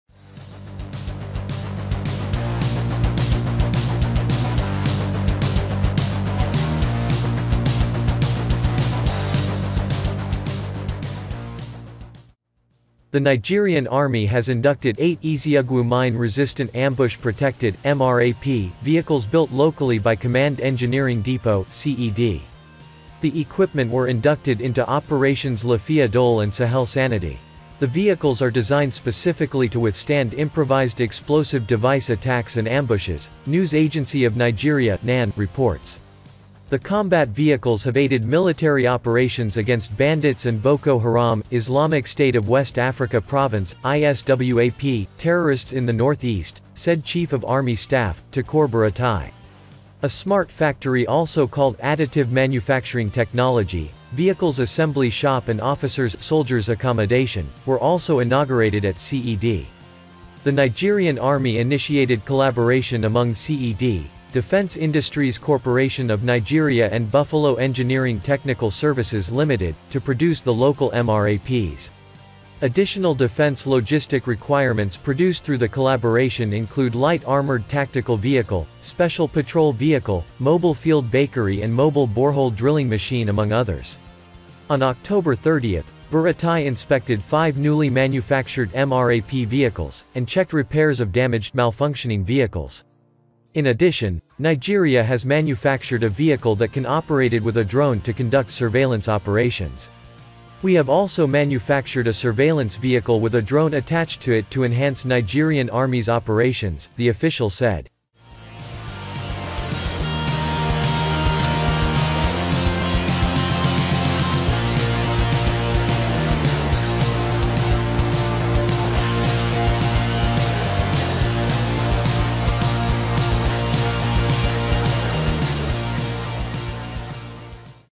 Nigerian Army has inducted eight EZUGWU Mine Resistant Ambush Protected (MRAP) vehicles
The Nigerian Army has inducted eight EZUGWU Mine Resistant Ambush Protected (MRAP) vehicles built locally by Command Engineering Depot (CED).

The equipment were inducted into Operations Lafiya Dole and Sahel Sanity.

A Smart Factory also called Additive Manufacturing Technology, Vehicles Assembly shop and Officers/Soldiers accommodation, were also inaugurated at CED.

The Nigerian Army initiated collaboration among CED, Defence Industries Corporation of Nigeria and Buffalo Engineering Technical Services Ltd., to produce the local MRAPs. Additional defence logistic requirements produced through the collaboration include Light Armoured Tactical Vehicle, Special Patrol Vehicle, Mobile Field Bakery and Mobile Borehole Drilling Machine among others.

On October 30, Buratai inspected five newly manufactured MRAP vehicles, and checked repairs of damaged/malfunctioning vehicles.

In addition, Nigeria has manufactured a vehicle that can operated with a drone to conduct surveillance operations. “We have also manufactured a surveillance vehicle with a drone attached to it to enhance Nigerian Army’s operations,” the official said.